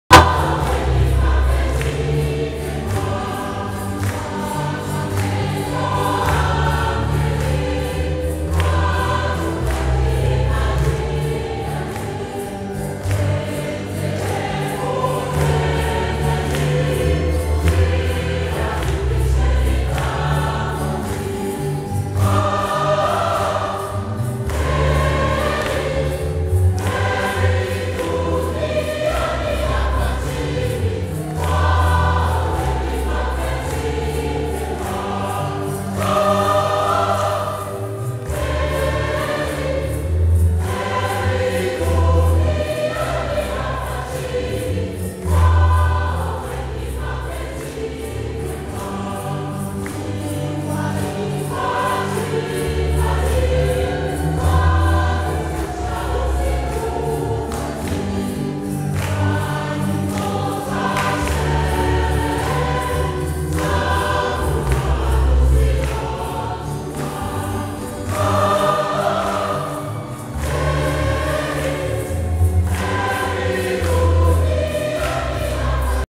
Echange des Voeux à la paroisse de Nguba
Procession d'entrée à la messe d'échange de Voeux par Mgr l’Archevêque de Bukavu